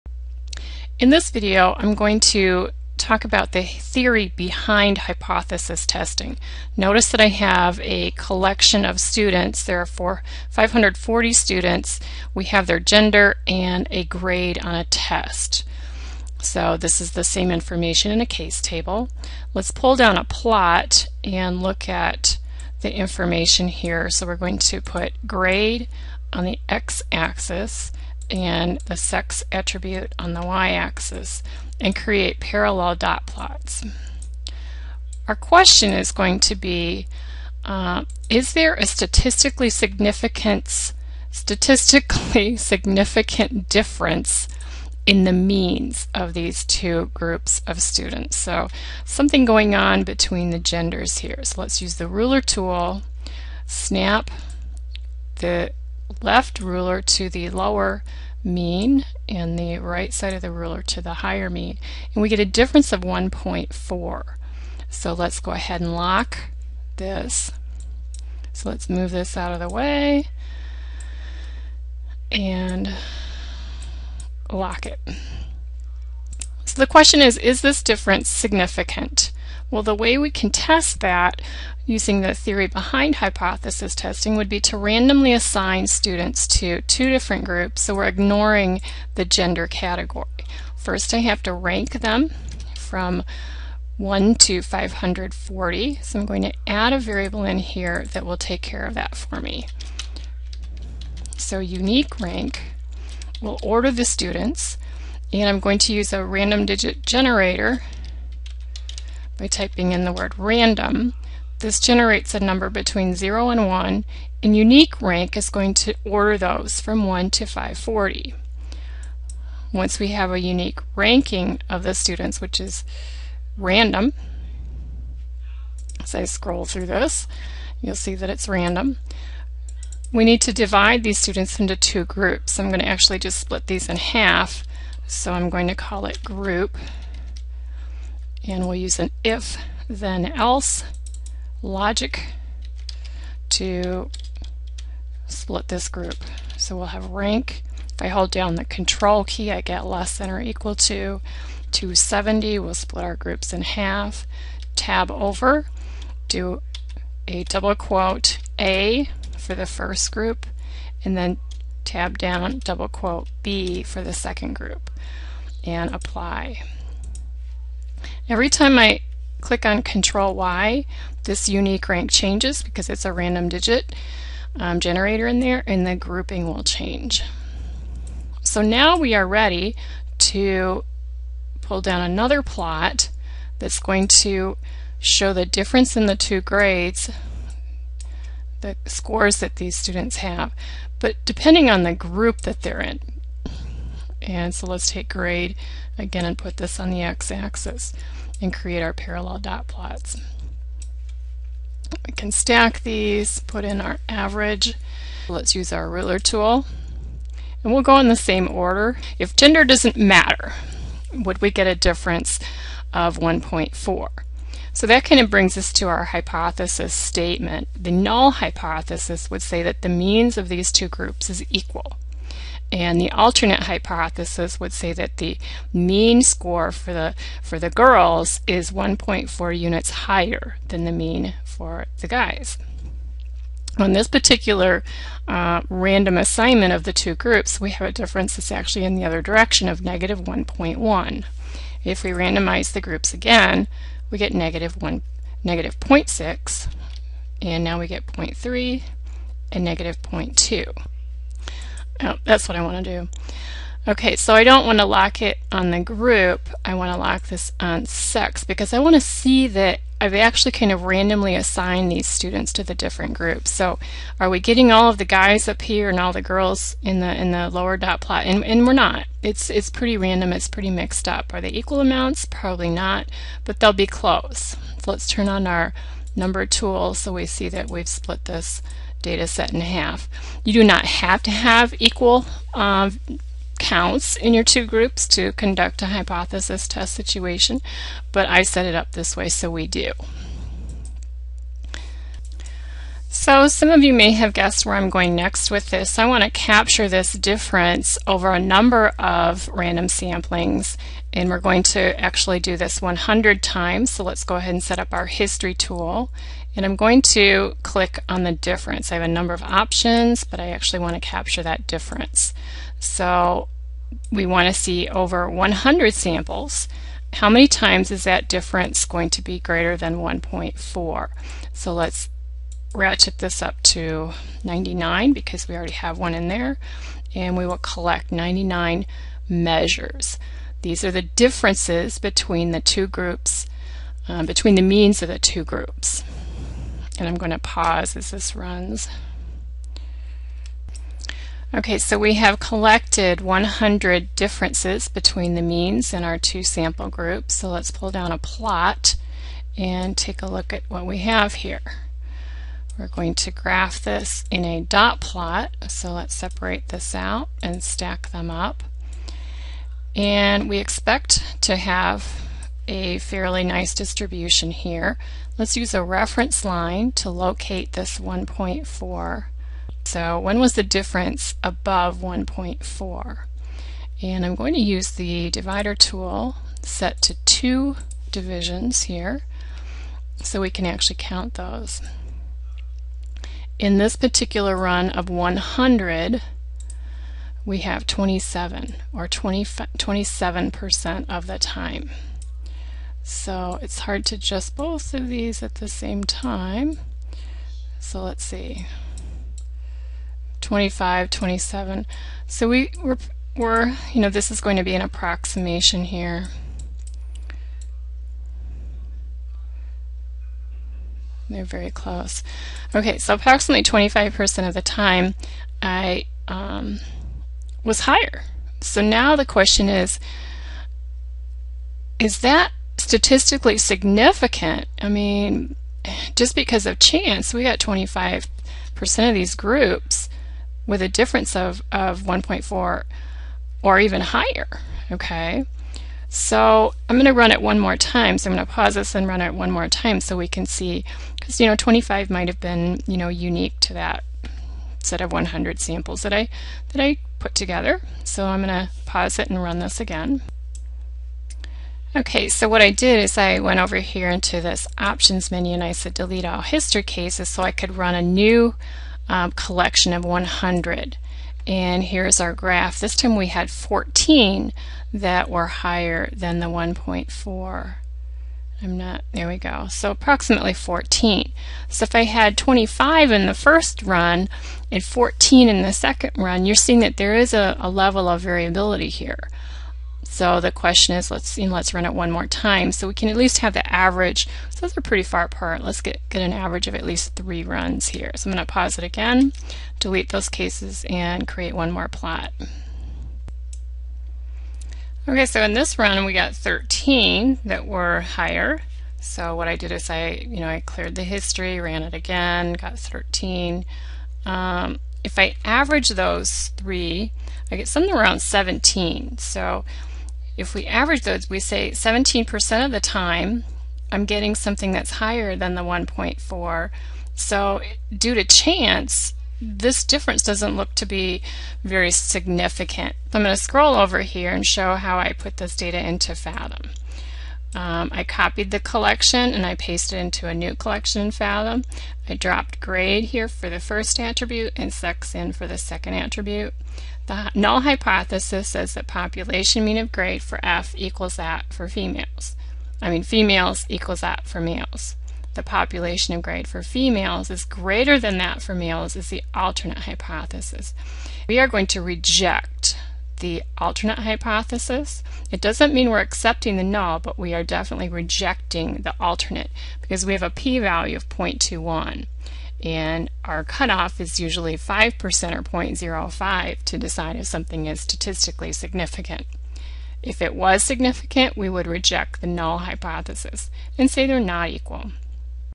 Hypothesis Testing in TinkerPlots
Compare the mean test scores based on Gender.  Is there a statistically significant difference?